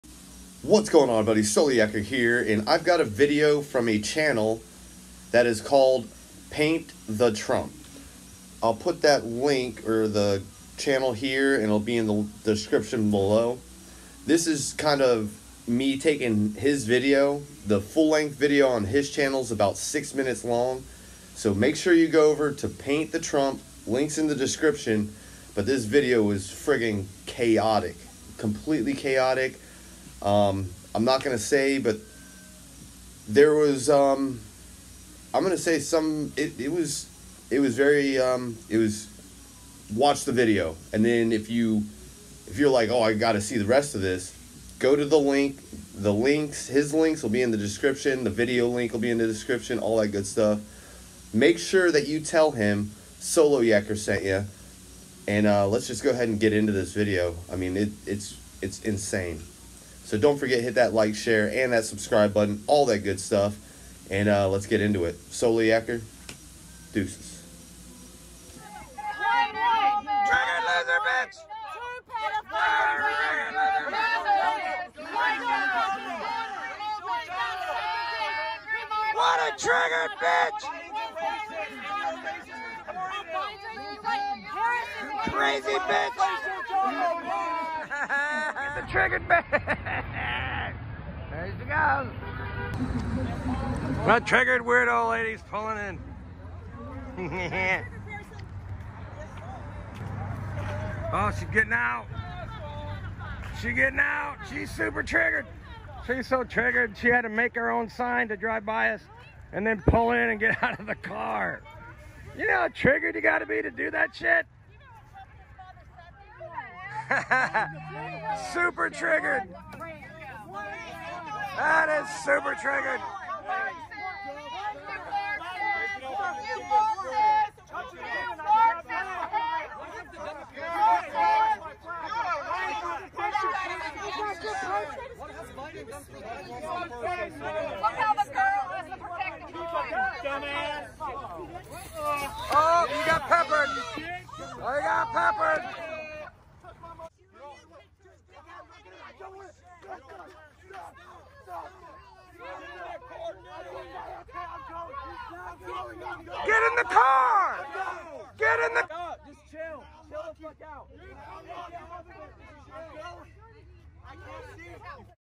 Complete Chaos!!! Biden Supporters Fight Trump Supporters!! Get Your Popcorn
Paint the Trump YouTube Channel

Original Video